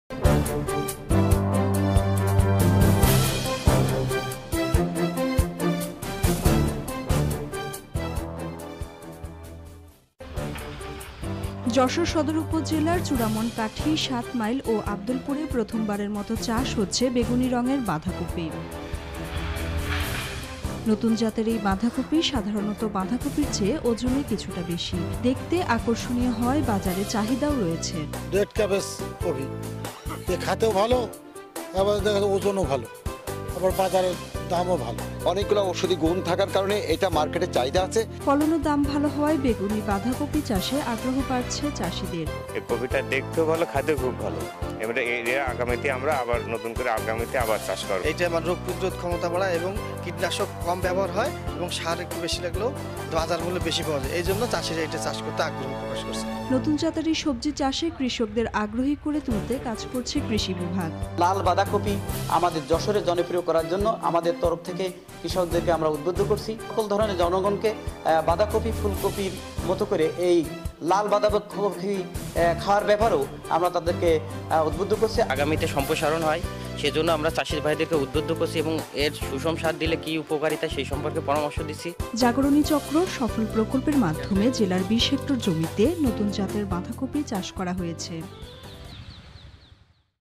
যশোর থেকে এবার ১৫ হাজার মেট্রিক টন বাঁধাকপি_Channel Bangla News 24 TV- on You tube
চলতি ও আগামী মাসে ইউরোপের চার দেশ সৌদি আরব, তাইওয়ান, সিঙ্গাপুর ও মালয়েশিয়ায় এ কপি পাঠাতে প্রস্তুতি নেওয়া হচ্ছে।